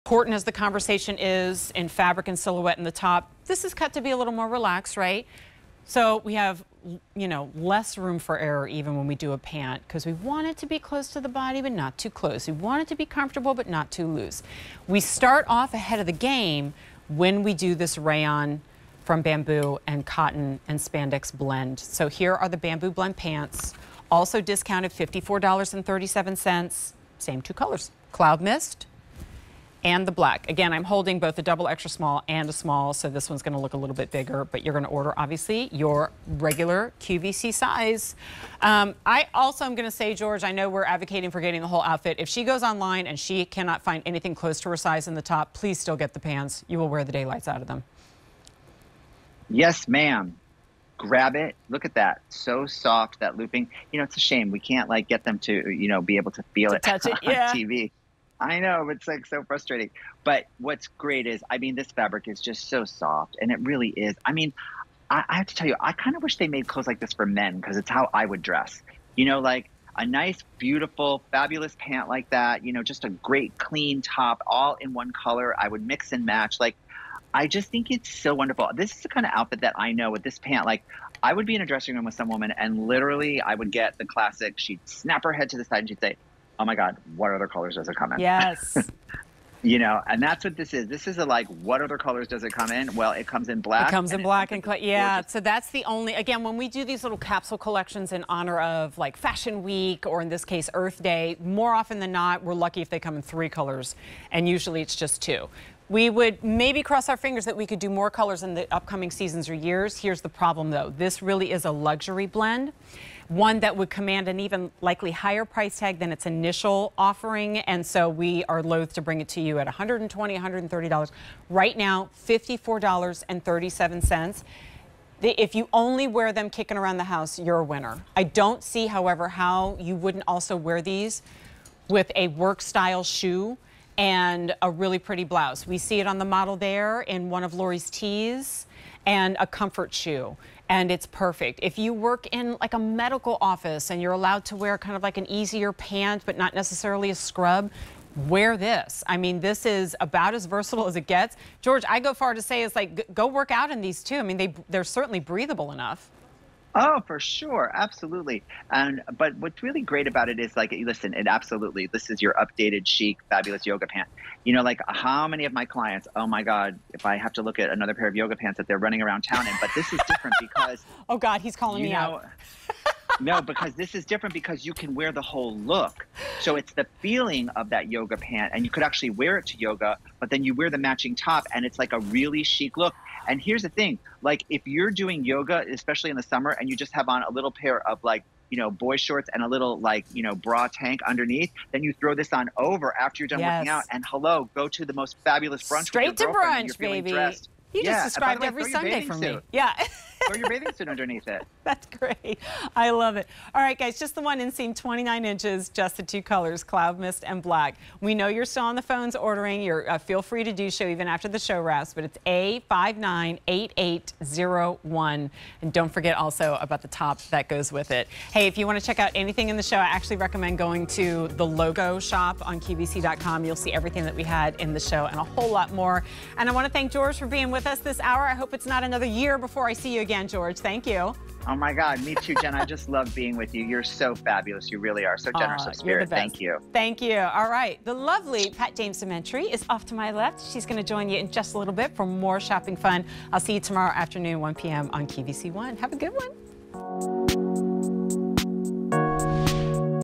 LOGO by Lori Goldstein Rayon made from Bamboo Blend Pants on QVC
LOGO by Lori Goldstein Rayon made from Bamboo Blend Pants

Ready for anything, these comfy pull-on pants exude everyday elegance. From LOGO by Lori Goldstein&reg;.